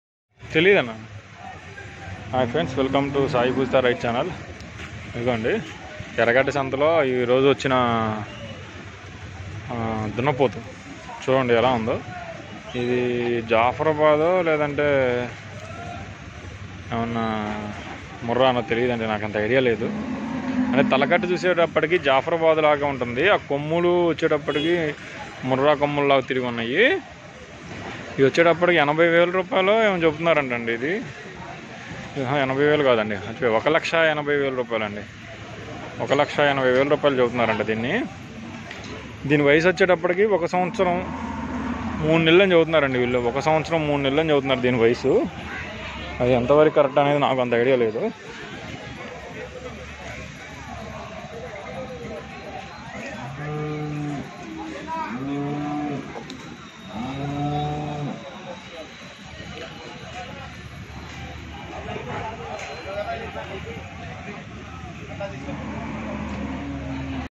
ఎర్రగడ్డ సంతలో దున్నపోతు
హాయ్ ఫ్రెండ్స్ వెల్కమ్ టూ "సాయి పూజిత రైతు ఛానల్" మీరు అందరూ ఎలా ఉన్నారు, అందరూ బావునారు అని ఆశిస్తున్నాను.

ఇపుడు నేను మీకు చూపిస్తున ఈ ఎద్దులు సంత, ఎర్రగడ్డ గ్రామము, అమీర్పెట్, హైదరాబాద్   తెలంగాణ రాష్ట్రంలో ఈ సంత ఉంటది అండి, ప్రతి ఆదివారం నాడు ఈ సంత జరుగుతుంది.

ఈ సంత చిరునామా:
ఎర్రగడ్డ గ్రామము,
అమీర్పెట్,
హైదరాబాద్,
తెలంగాణ రాష్ట్రం.

Hi friends, welcome to sai poojitha rythu channel, how are you all, I think you are all good, now I am showing you when the bulls market comes, when this bulls market in Telangana comes near Hyderabad, erragada village, this bulls market every sunday these bulls  this bulls market is mostly bulls and cow's and buffelos belonging to the state of Telangana.

In the state of Telangana, bulls are mostly found in this bull market, When it comes to pairing, the rate is around Rs 1 lakh to Rs 3 lakh,Most of the bulls and cows of Telangana state are found in this herd of erragada.

Address:
erragada is a Village in erragada Mandal in Hyderabad of Telangana State, India. It belongs to Telangana region .